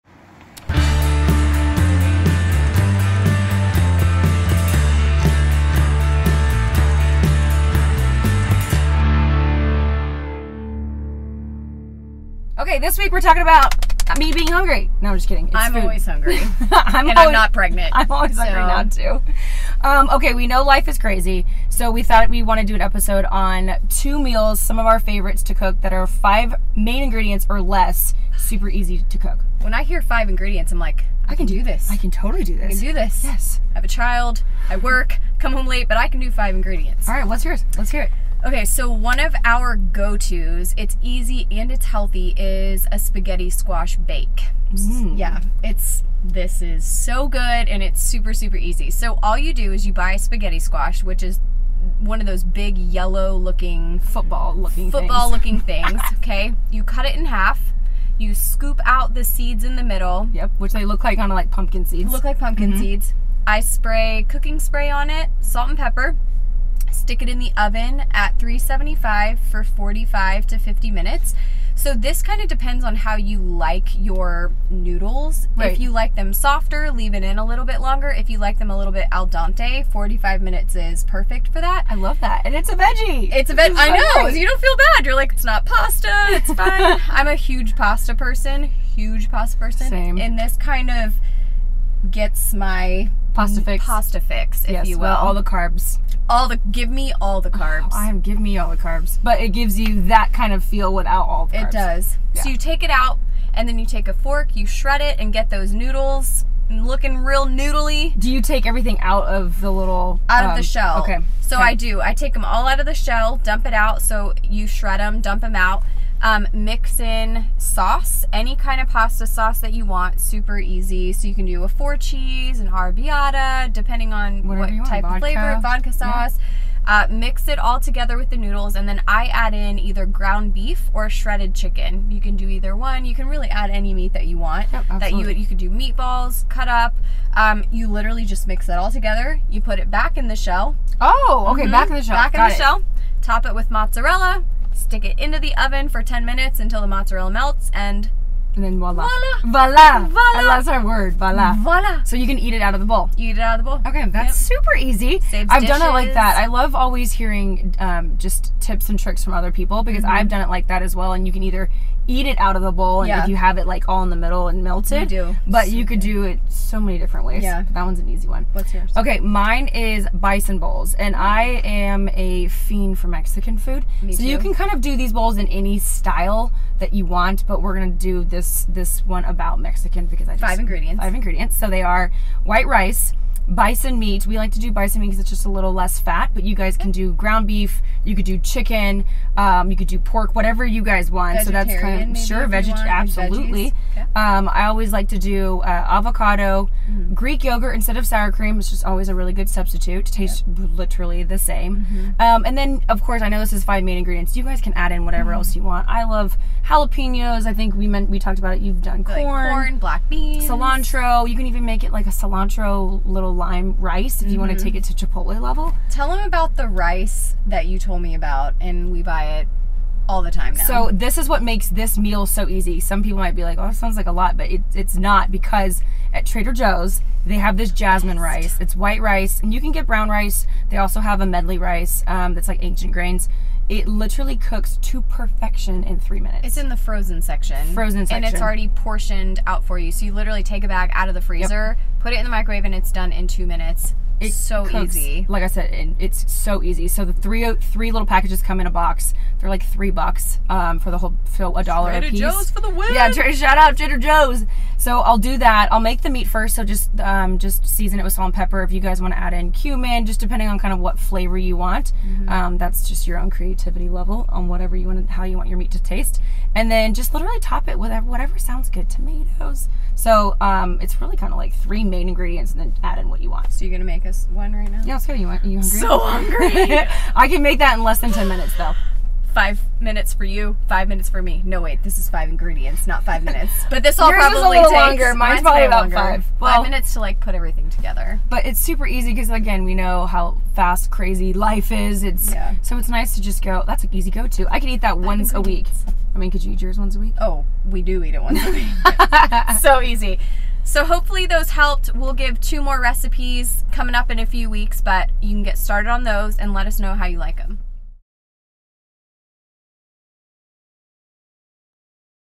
5 Ingredient Dinners!
We all know life is CRAZY busy...work, kids, events, activities, etc, and figuring out what to eat for dinner can be a total beat down. With that being said, we wanted to share with you some of our easy
go-to dinner recipes with only 5 main ingredients or less, all while staying within that healthy eating zone. This week we’re talking about a couple meals that will hopefully make your life a tad easier! We love to hear from you too, so if you have a recipe please share it in the comments below. We are in this together!